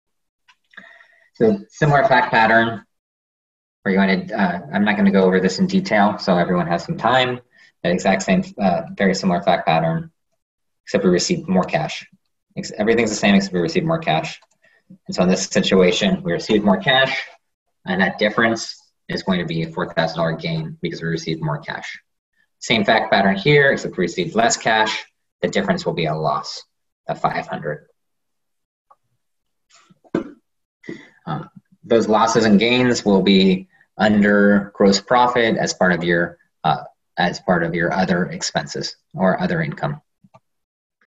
Selling Plant Assets Above Book Value - Journal Entries Gain and Loss
Devon Coombs, CPA, discusses accounting for PP&amp;E and long term assets accounting.